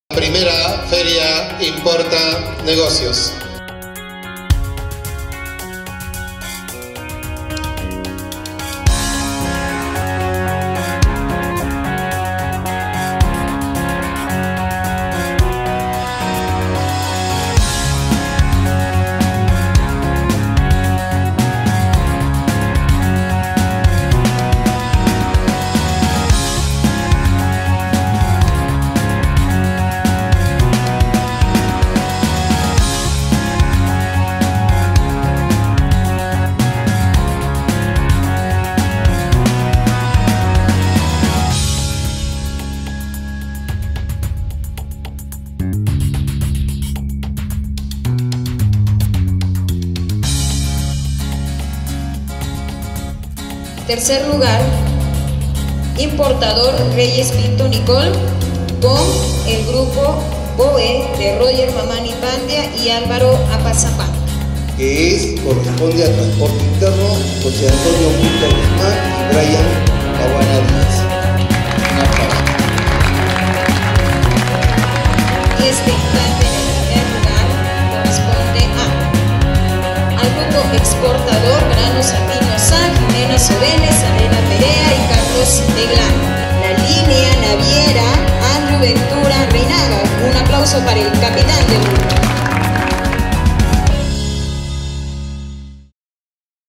Feria Importa Negocio 2017
I Feria “Importa Negocios 2017”, organizada por la escuela profesional de Administración de Negocios Internacionales, donde se presentaron artículos importados por alumnos del I ciclo desde el continente asiático a través del portal web  arequipaexpress.com.
Además se realizó el concurso sobre el circuito de exportación e importación de productos, siendo los ganadores el grupo de Exportador Granos Andinos S.A.C y el grupo Linea Naviera.